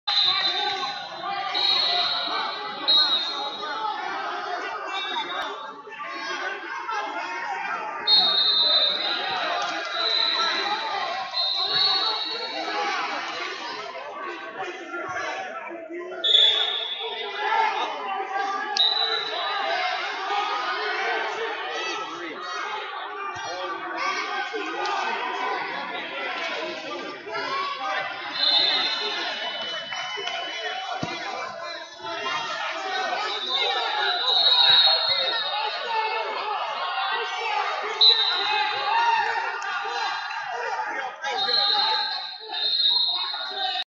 HS Girls 152 Emma Thompson Oklahoma Vs Cadence McCullough Southern Arkansas Wrestling Academy
2021 Southern Regionals GR - HS Girls 152 Emma Thompson Oklahoma Vs Cadence McCullough Southern Arkansas Wrestling Academy